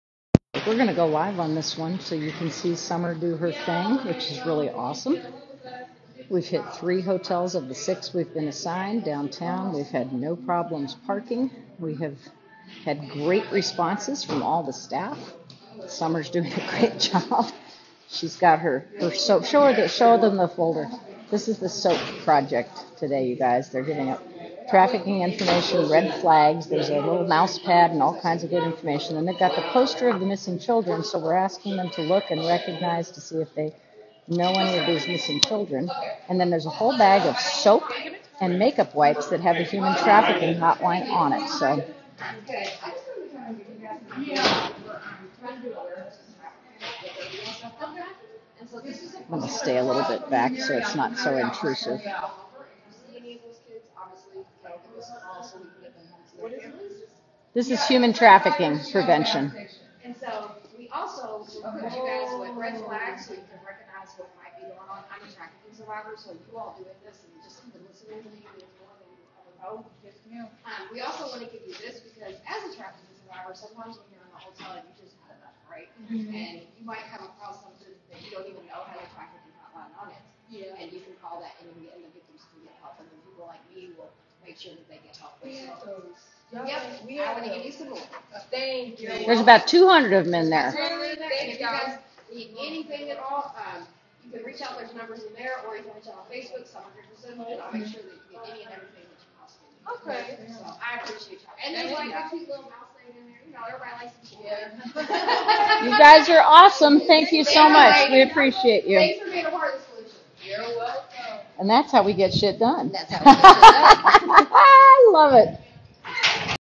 Summer Dickerson of Women of the Well Ministry and @502LIVEstreamers working SOAP Project outreach1
Saving Our Adolescents from Prostitution (The SOAP Project) is a nationally renowned organization battling human trafficking, particularly at the big money events that attract the demand for buying and selling of adults and children. What better partner in Kentucky than Summer Dickerson of Women of the Well Ministry and @502LIVEstreamers, who presented at  the training of 300 volunteers! All the hotel-bound soap and makeup wipes were then labeled with the National Human Trafficking Hotline number,  Teams scattered across the city on this first day of Derby Festival to inundate hotels with persuasive introductions, posters of missing children,  "red flag" information folders, and the supplies that housekeeping can leave in  rooms for potentiallly-trafficked victims to discover the little lifelines.